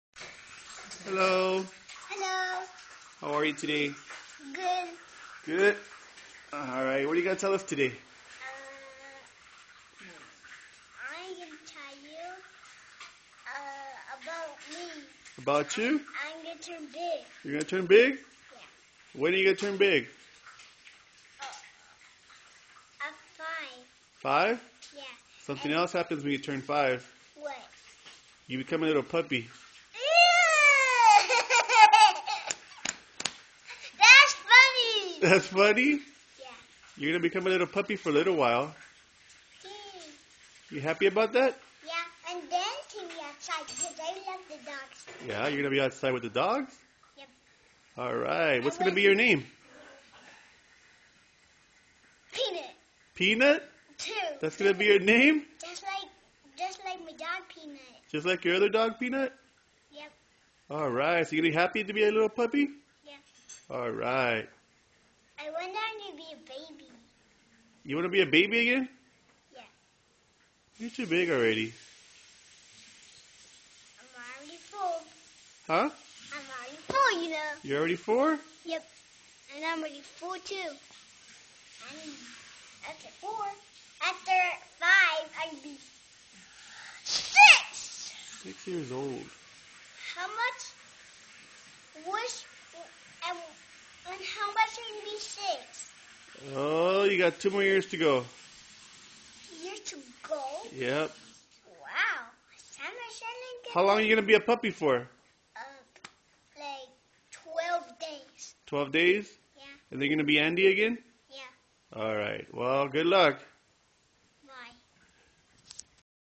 Funny Reaction interview Jimmy Kimmel w/ 4yr old
Funny Reaction...interview with my son...happy about his upcoming birthday